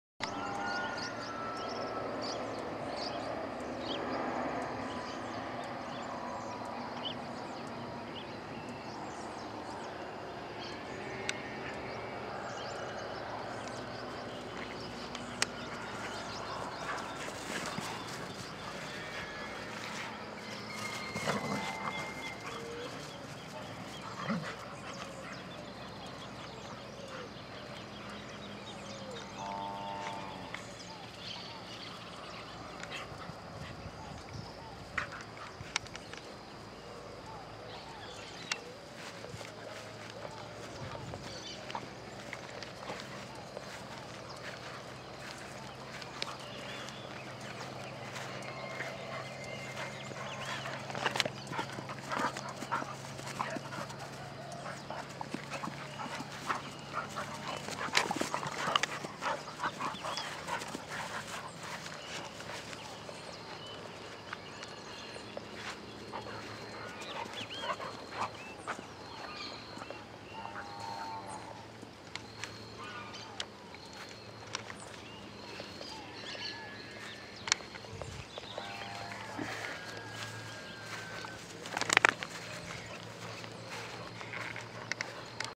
JVC GZ-HM440AU Test
Testing the JVC camera in the morning light